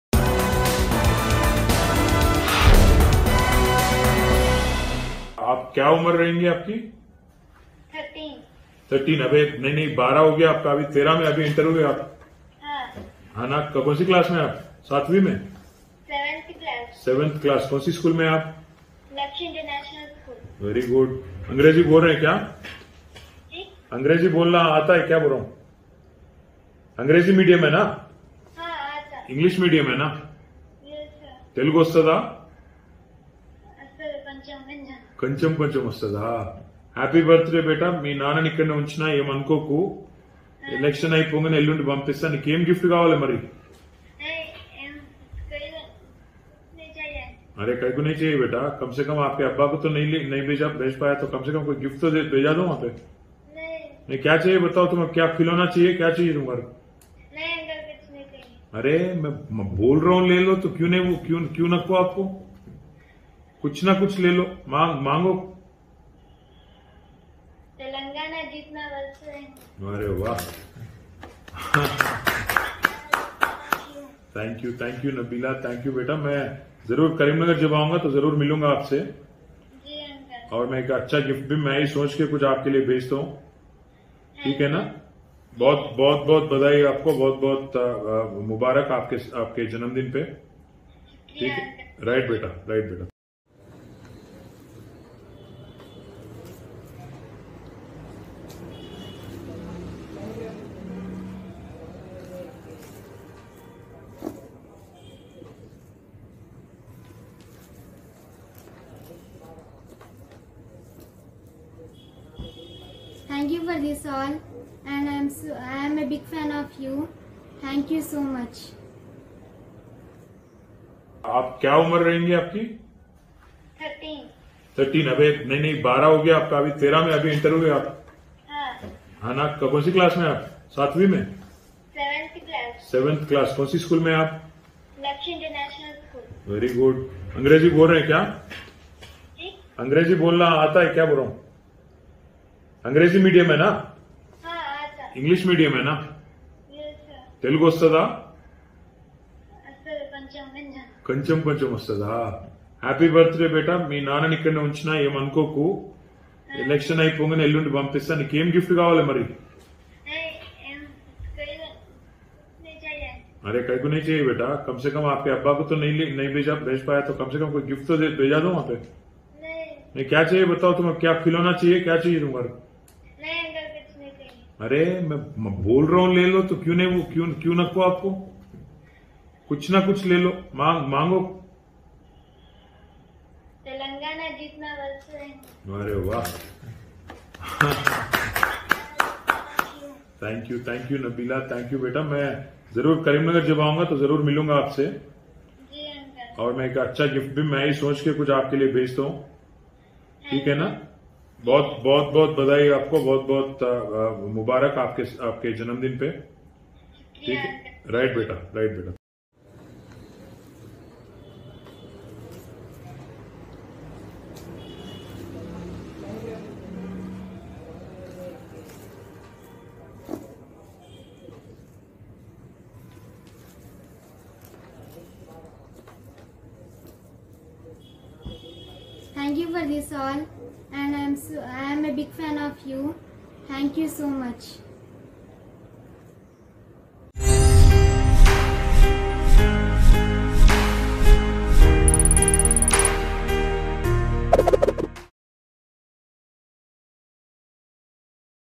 Minister KTR Conveys Birthday Wishes To TRS Activist's Daughter | Telangana Politics | Mango News
Minister KTR Conveys Birthday Wishes To TRS Activist's Daughter.